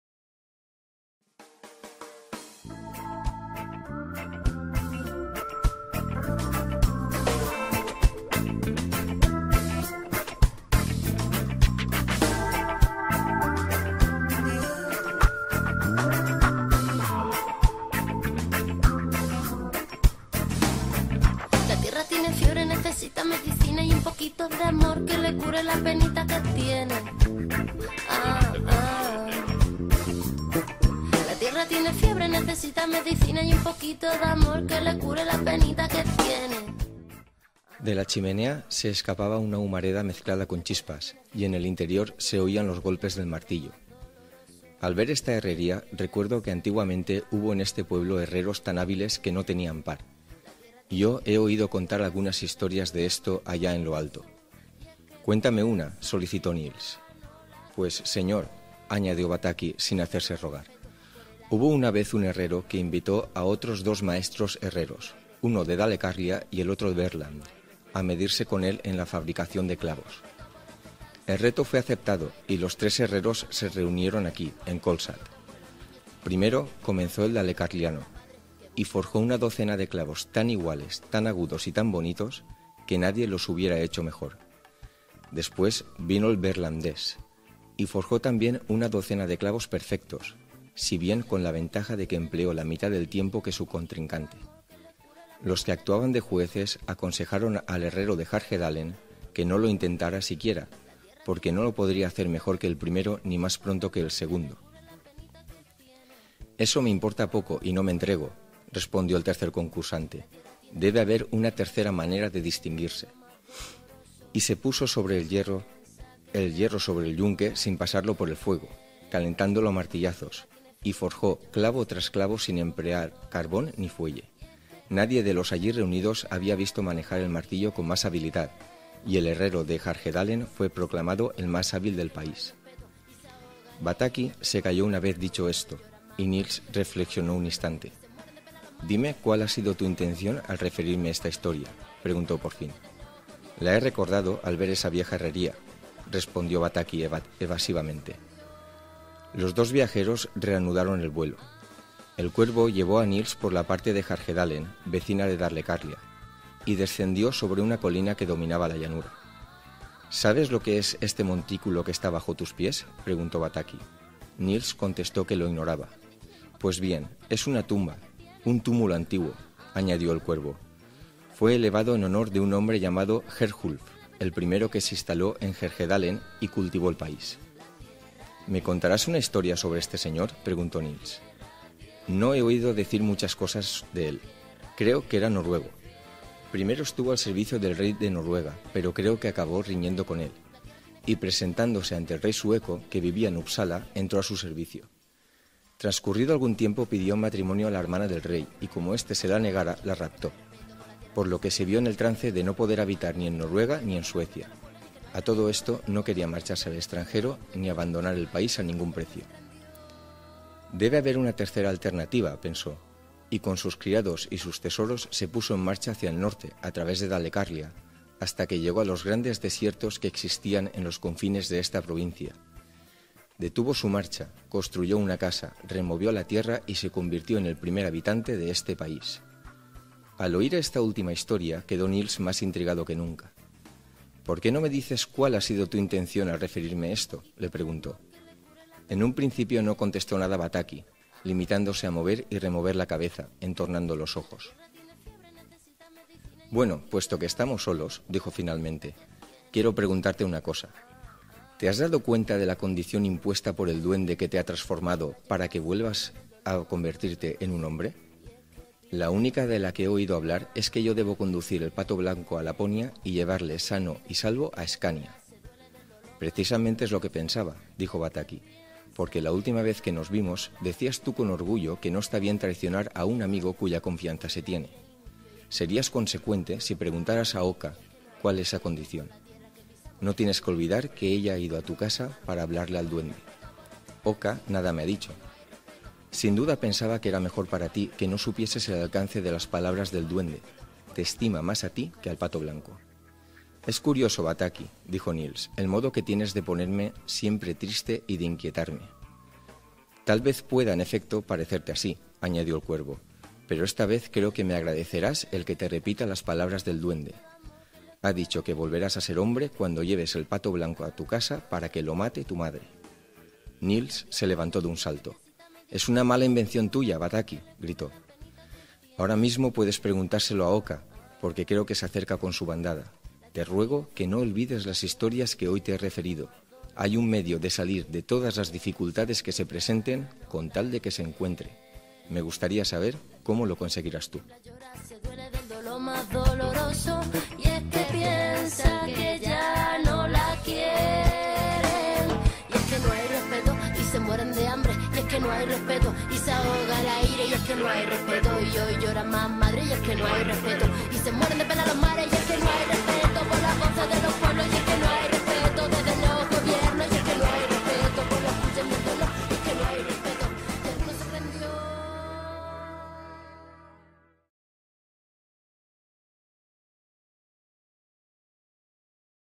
El Maravilloso viaje del pequeño Nils - Regalo literario de Nando Pilgrim
Hay deseos que anidan desde siempre en la condición humana y especialmente en los niños que crecen, de cualquier lugar y tiempo.

En cuanto los leas, tal vez reconozcas que también son o han sido tuyos: poder volar, escapar de casa y decidir sobre tu vida, tener un amigo que te comprenda, viajar a lugares lejanos, confiar en alguien sabio, tener poderes para castigar a los necios y ayudar a los honestos, ser útil y que te valoren…

En el viaje que hacemos con este clásico de la literatura infantil y juvenil, no falta ni uno solo de estos deseos. ¿Nos acompañas?